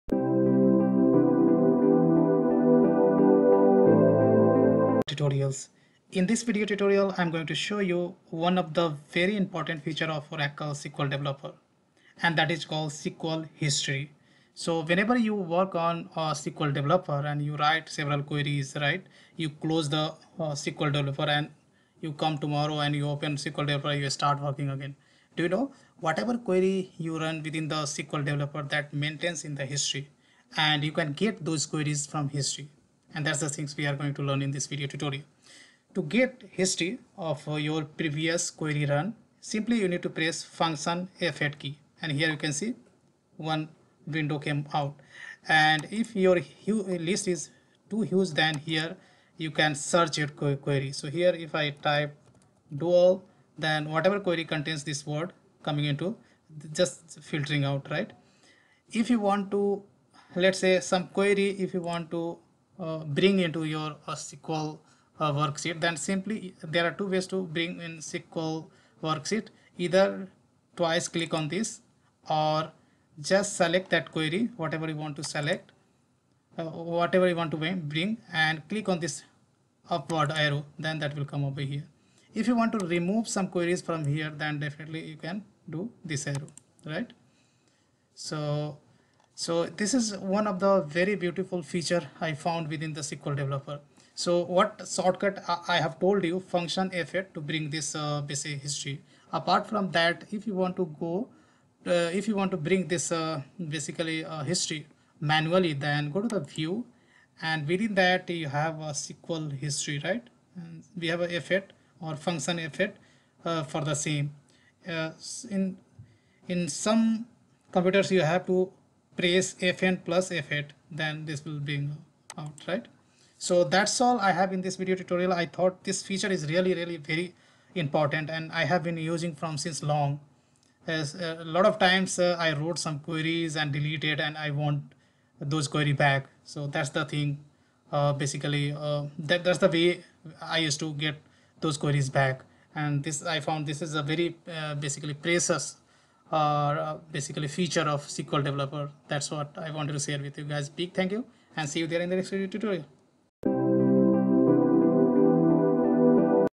SQL History tips and tricks in Oracle SQL Developer? || Oracle SQL Developer Tutorial
About this Video:
In this video, We will learn about SQL History tips and tricks in Oracle SQL Developer ?